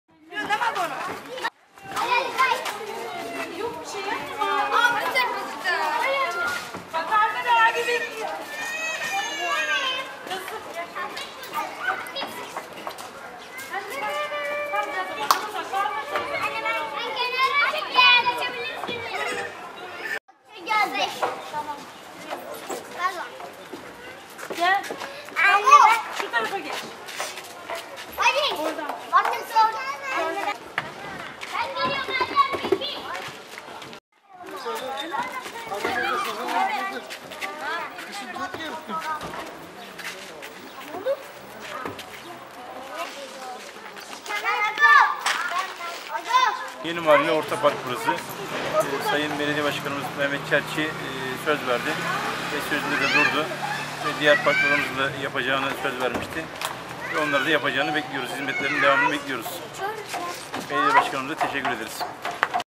Yunusemre Belediyesi Sözünde Durdu
Yunusemre Belediyesi daha önceden sözünü verdiği projeleri  hayata geçirmeye devam ediyor. Yunusemre Belediye Başkanı Dr. Mehmet Çerçi, hafta sonu incelemede bulunduğu sırada yapılması talimatını verdiği  Yeni Mahalle Orta Parkı, iki gün içerisinde hayata geçti.